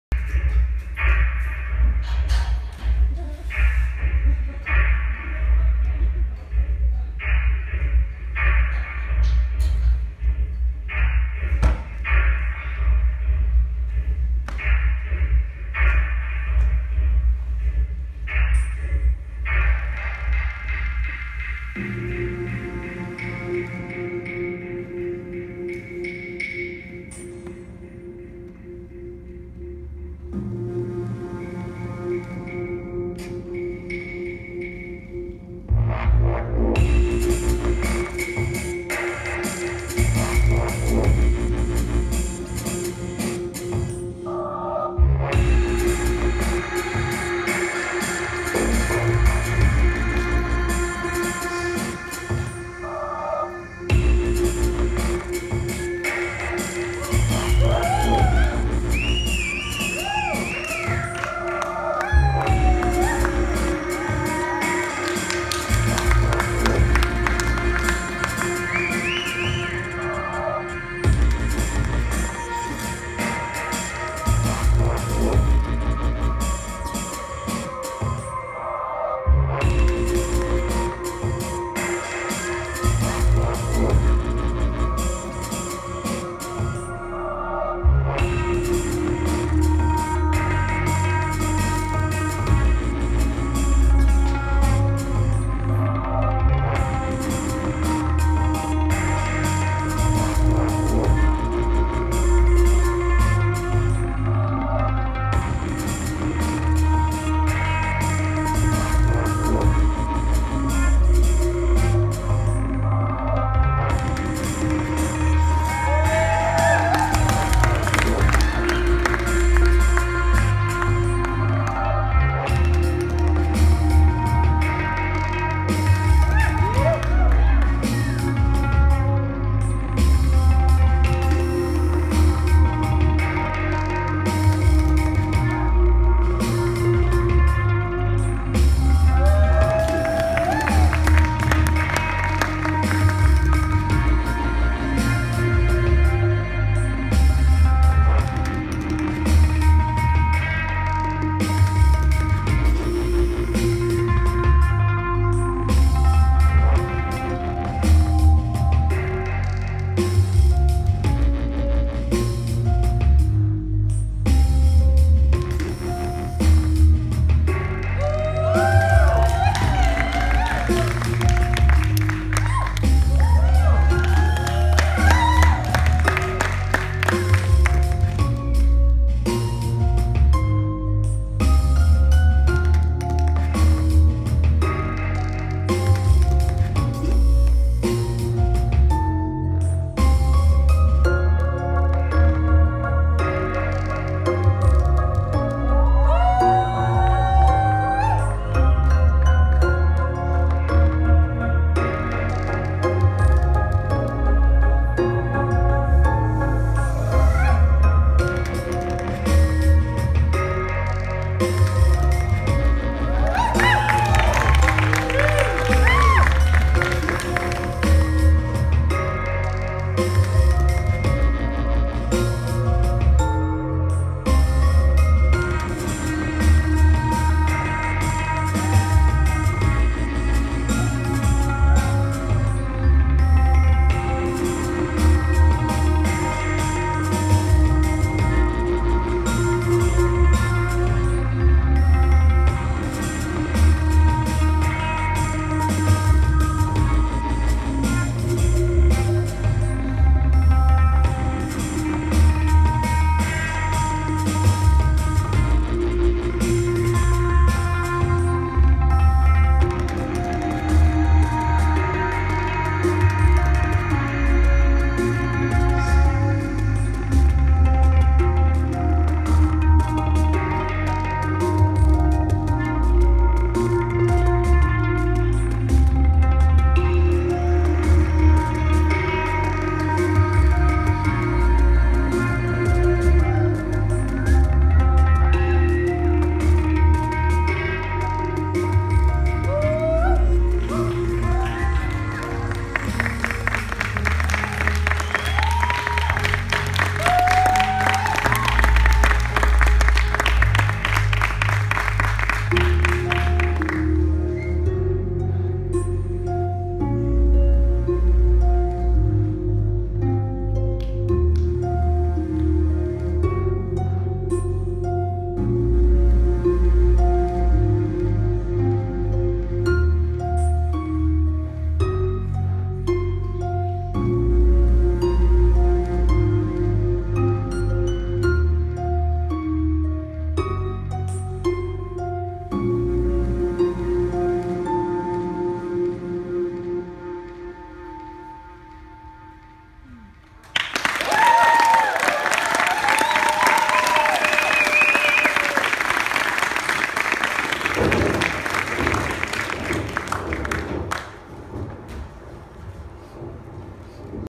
Natalie Rhae updated version
Natalie Rhae swinging pole from salvage circus updated version